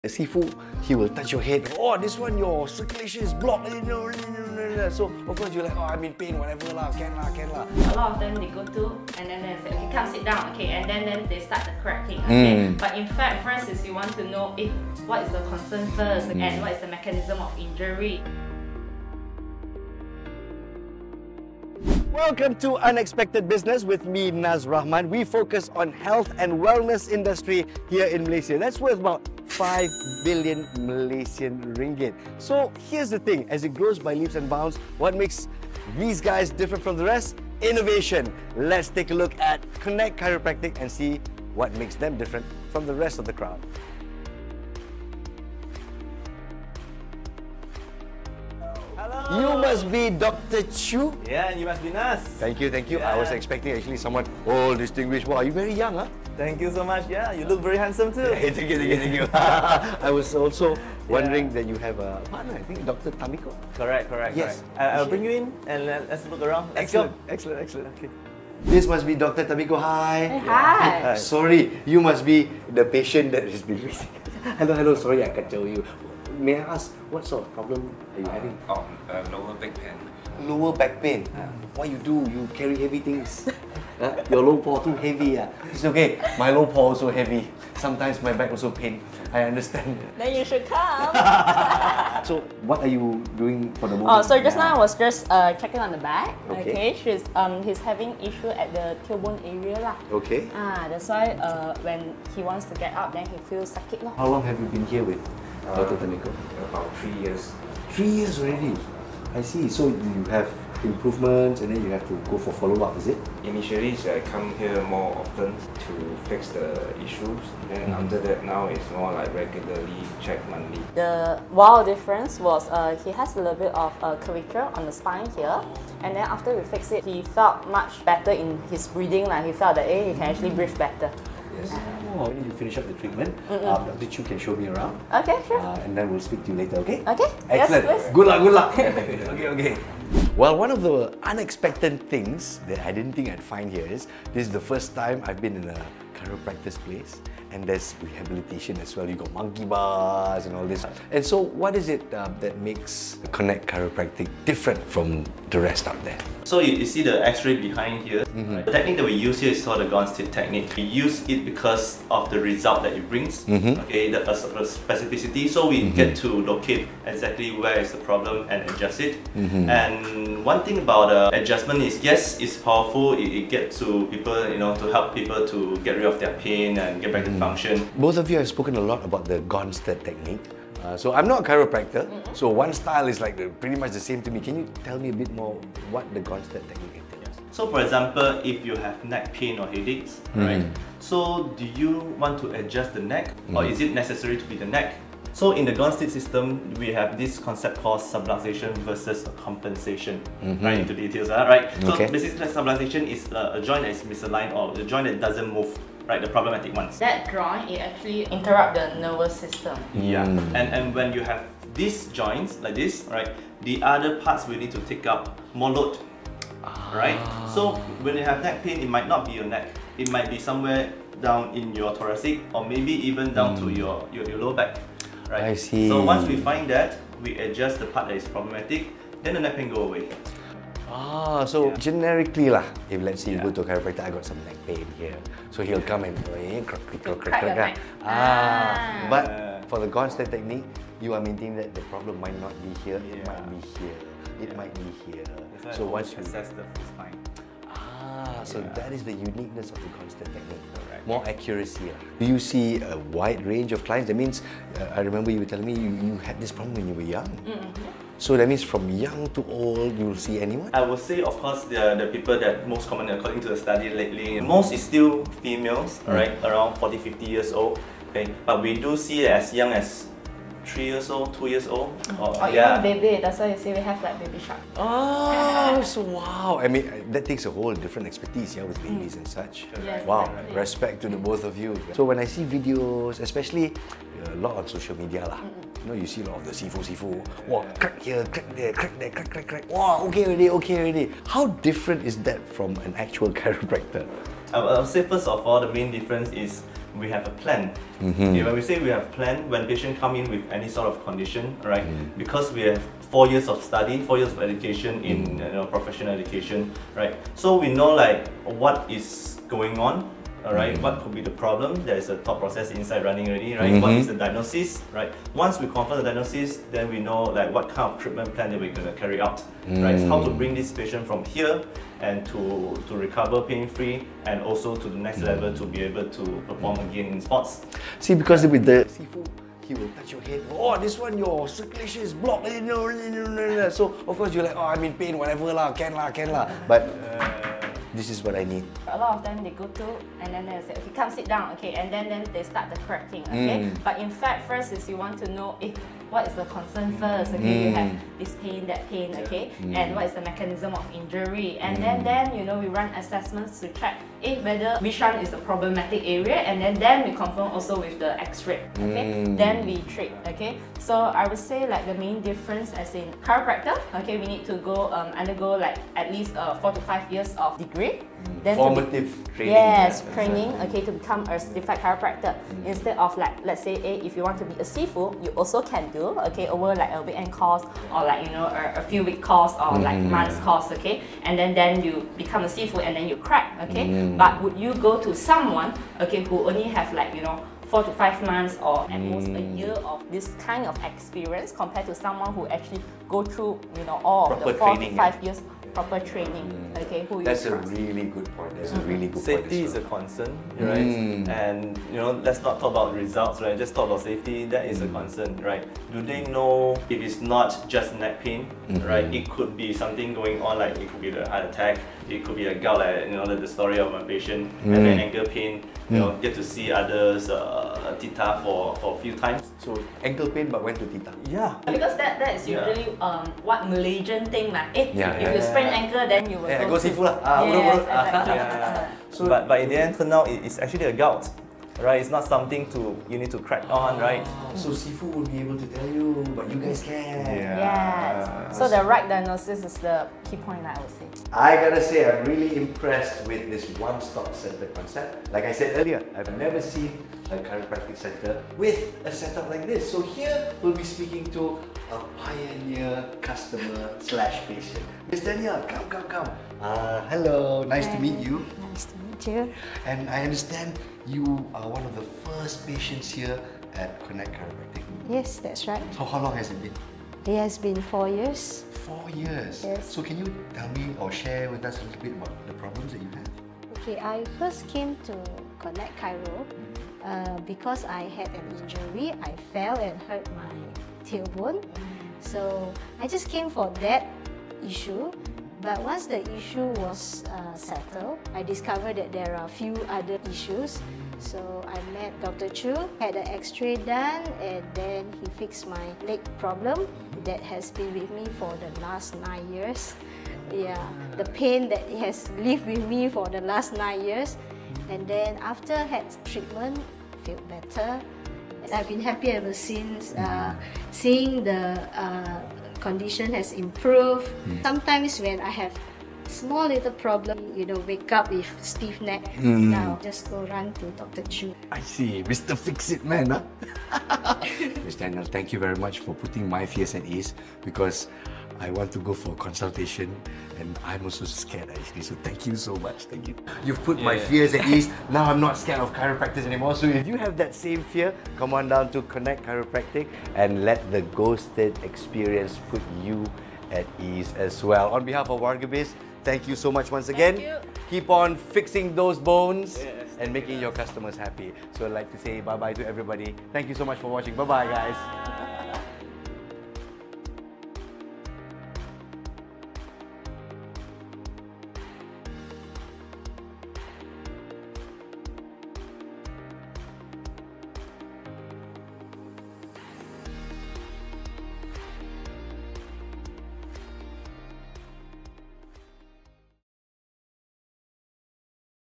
Unexpected Business: Connect Chiro founders talk about chiro & physio care, host by Nazrudin
Unlock the full potential of a pain-free life! We chat with the experts at Connect Chiropractic on their unique approach to holistic spine and joint health.